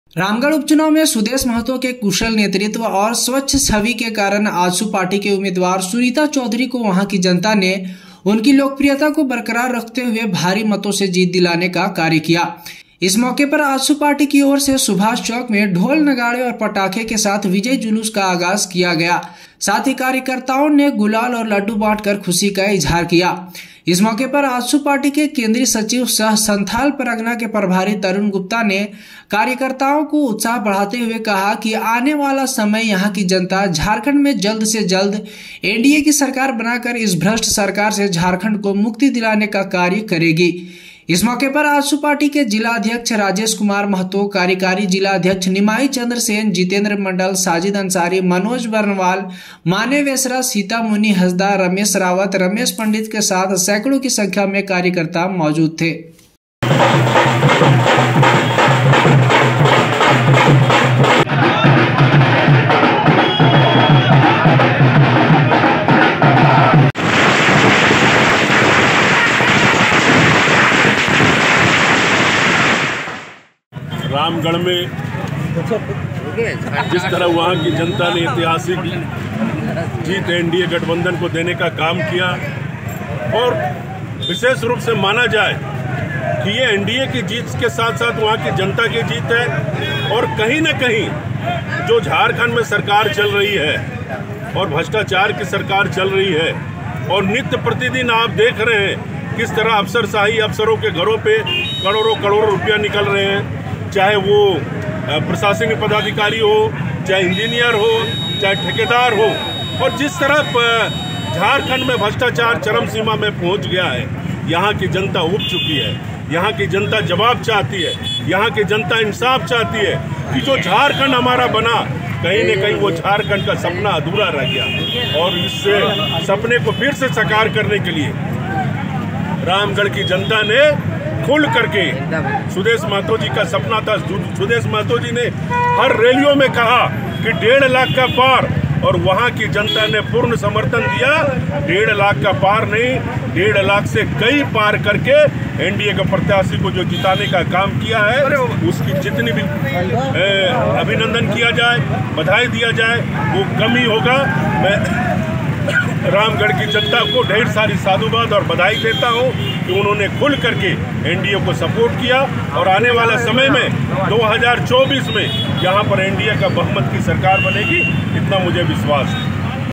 आजसू ने आखिरकार रामगढ़ में लिया बदला, रामगढ़ उपचुनाव में भारी मतों से NDA की जीत
धन्यवाद।